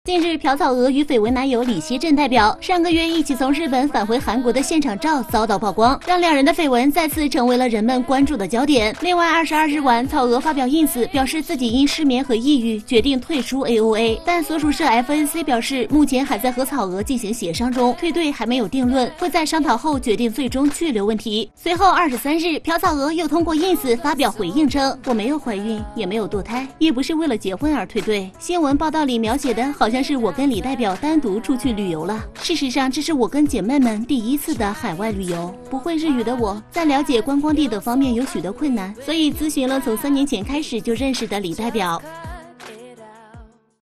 【3年前】AOA草娥跟富商約會照曝光 回應稱未懷孕或流產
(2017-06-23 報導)

AOA成員草娥被曝上月與緋聞男友同游日本。而近日，樸草娥與緋聞男友李錫振代表上個月一起從日本返回南韓的現場照遭到曝光，讓兩人的緋聞再次成為了人們關注的焦點。